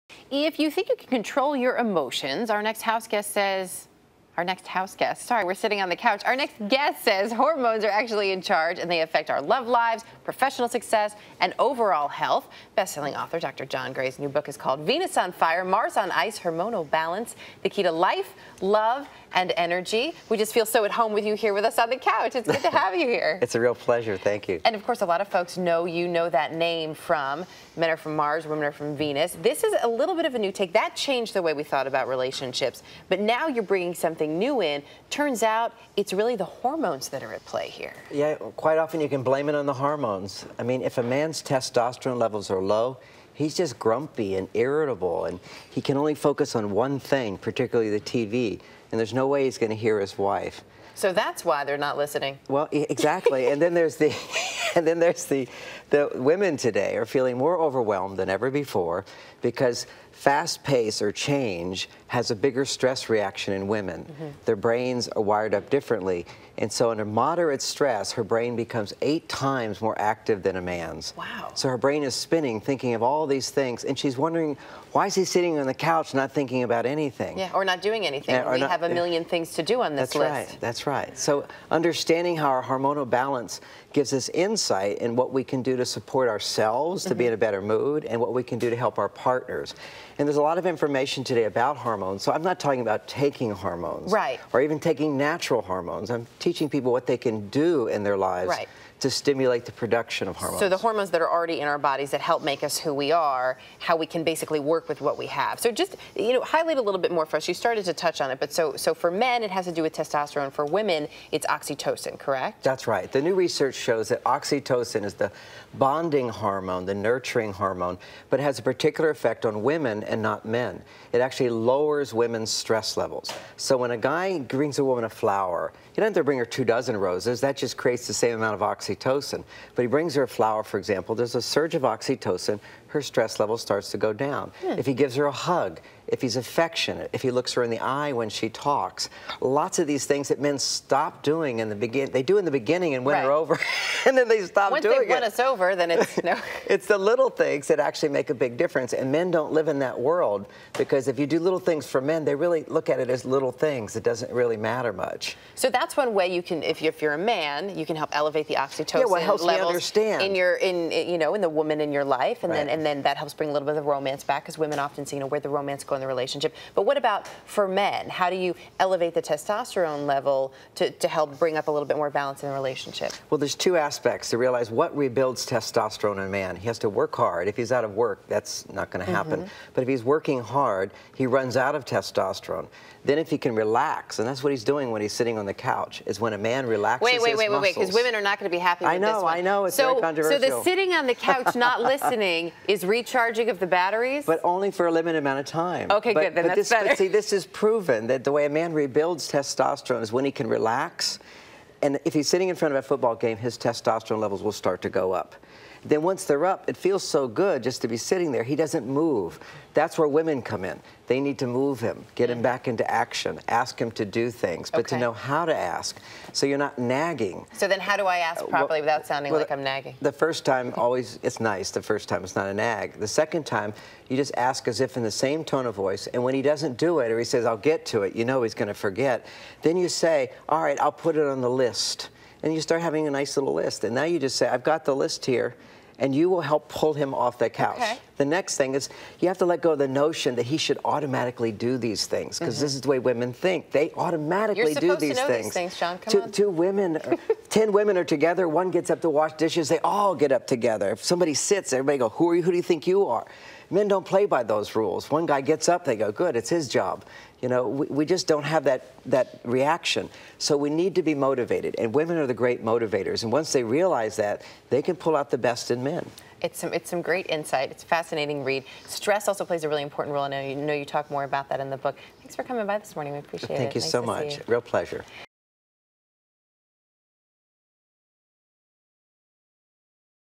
Super-stimulate your hormones with Dr. John Gray
Erica Hill talks to John Gray, PhD, relationship expert and author of "Venus on Fire, Mars on Ice" about how hormones affect our relationships.